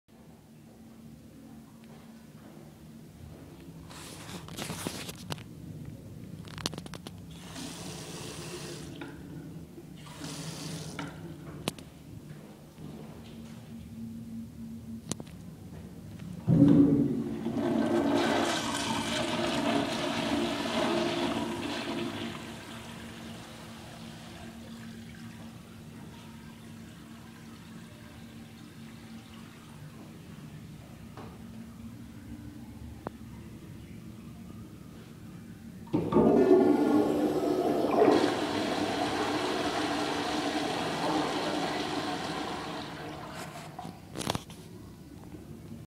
[403] New AS Madera and Eljer Correcto (Washdown)
-At a Jack-In-The-Box Restaurant

Nice fixtures, not particularly a fan of washdown urinals, but it does the job.  The madera could be stronger.